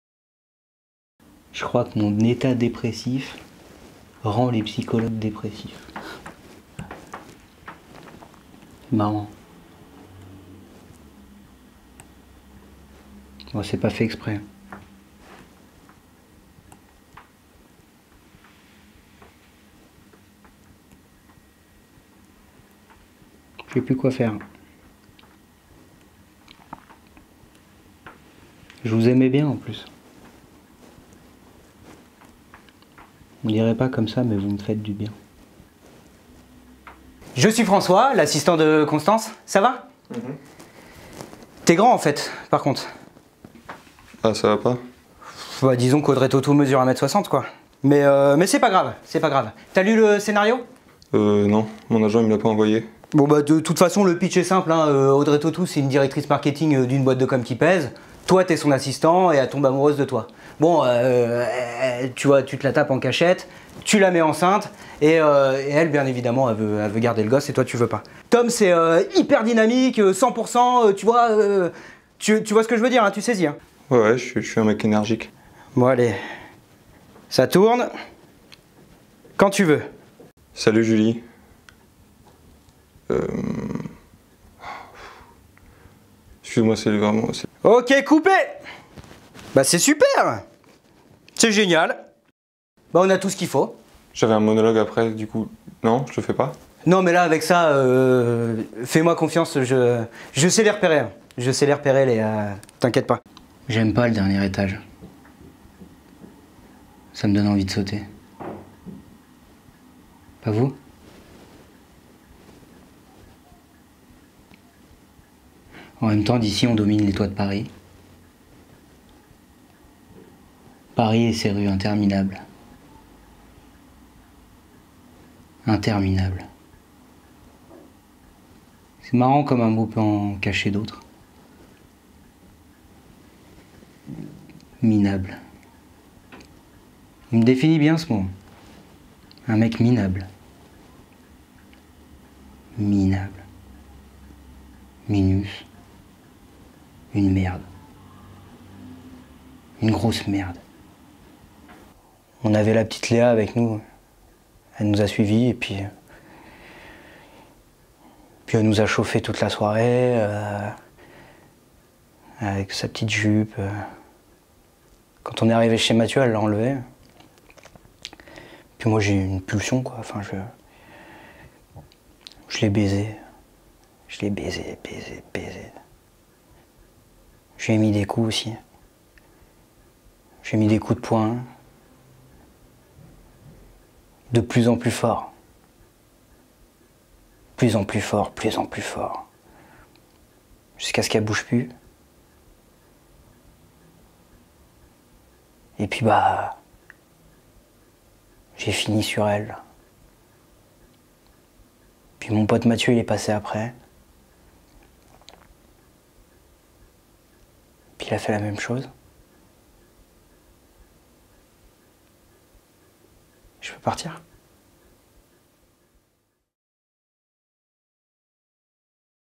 Bande démo 2021 - Valentin Daugeron - Comédien
Bande démo 2021 de Valentin Daugeron réalisée par Kevin Laméta

Comédien de 28 ans, ancien sommelier passé par:

- la troupe de théâtre Avenue du spectacle
- Formé par Laurent Charpentier, Antonia Malinova, David Garel et d'autres
- Membre du trio ValEDDam (avec Eddy Moniot et Damien De La Rochebrochard)

- Actuellement à l'écriture d'un seul en scène